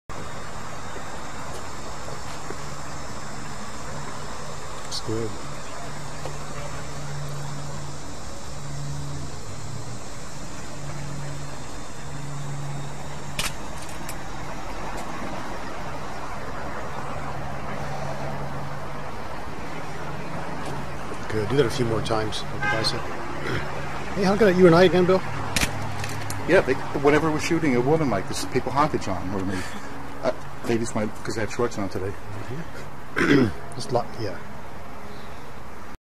Jennifer Abrams
She had a unique physique as at 5'6" 150 pounds one would think she would be at off-season weight, but she was very defined all over and with some of the deepest cut abs ever- the type normally seen on the more lean women competitors.